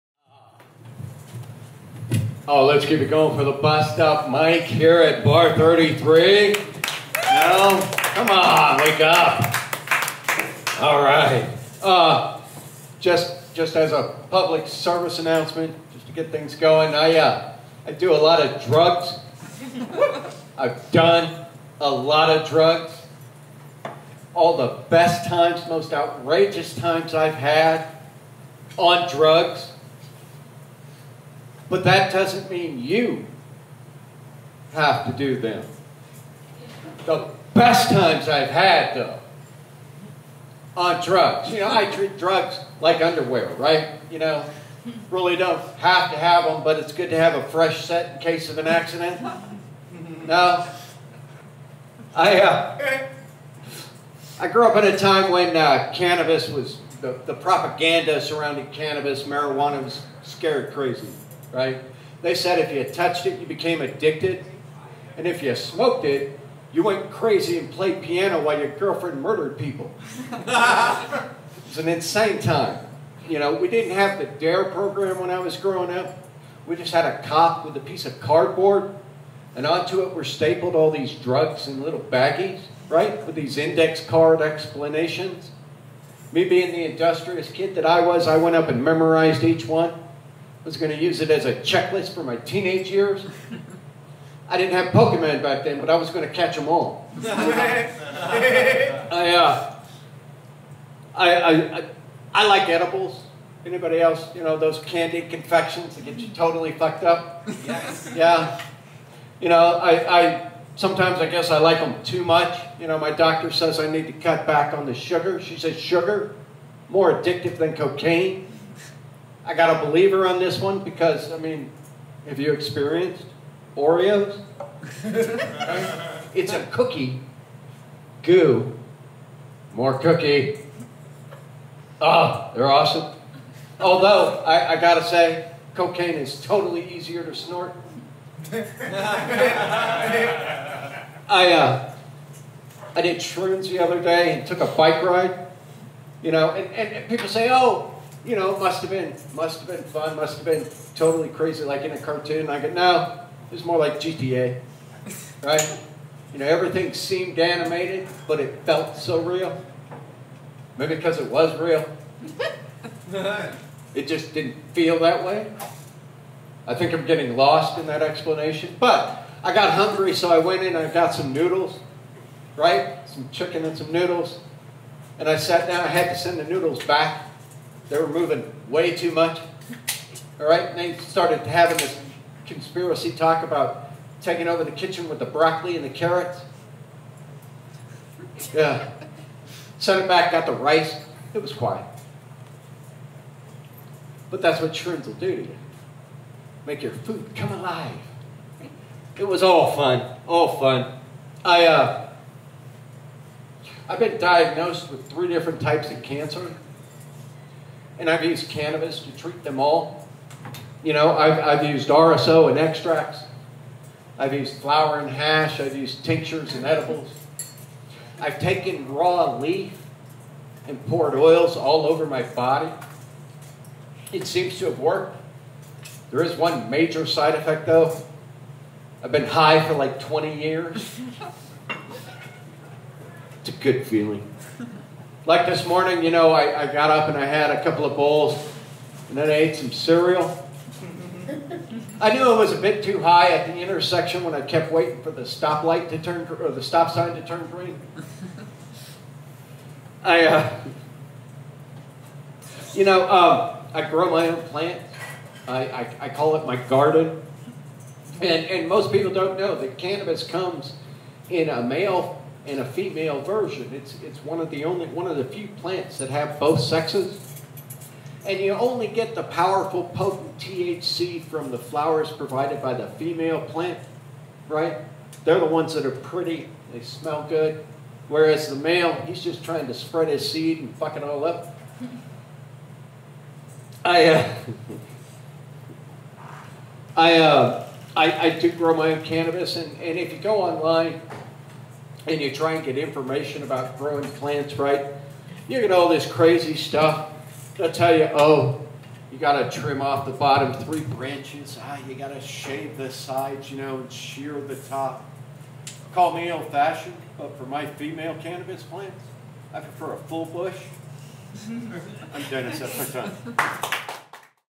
Dennis Cruise Bar 33 10 9 2019
Cannabis Set at Bar 33 - Interrupted by Ambulance during on joke